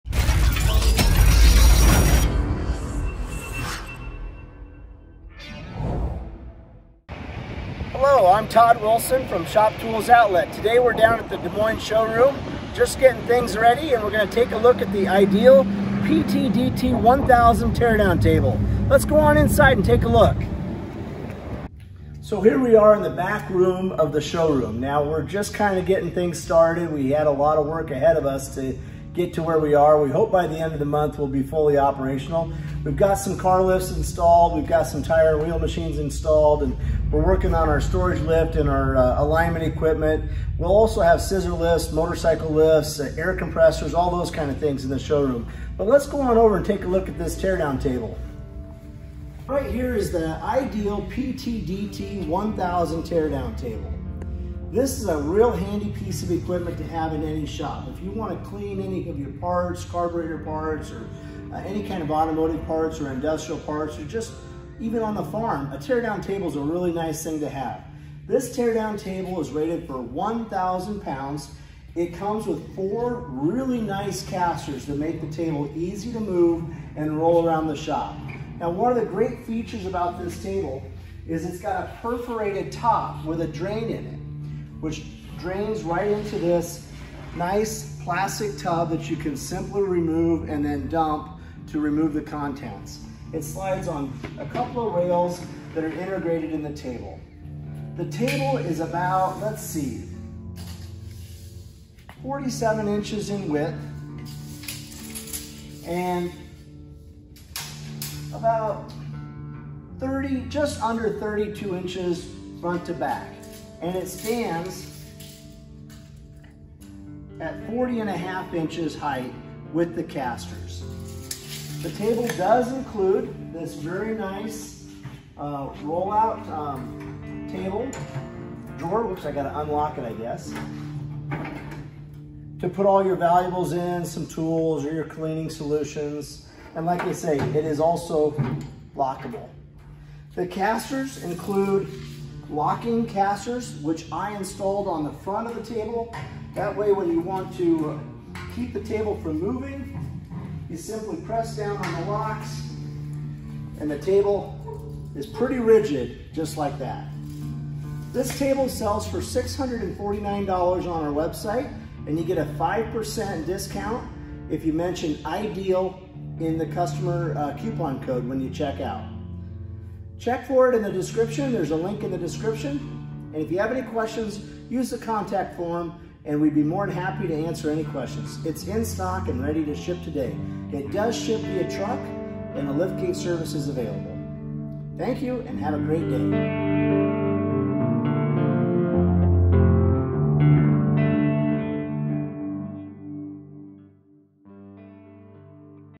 IDEAL Tear Down Table PTDT-1K
The Ideal®| PTDT-1K Tear Down table is one of those must-have tables for any shop.  With an integrated parts cleaning basin, this table lets you clean, inspect, and repair mechanical parts.  At less than 200 lbs. this PTDT-1K tear-down table ships to both residential and commercial addresses.  It is an excellent and affordable choice for any home garage or shop.  Assembly takes less than an hour with a bolt-together design.

FEATURES AND BENEFITS:
Heavy-duty steel construction
Bolts together for quick assembly
Integrated table drain with catch basin
Large 46″ x 31″ table
Blue powder coated finish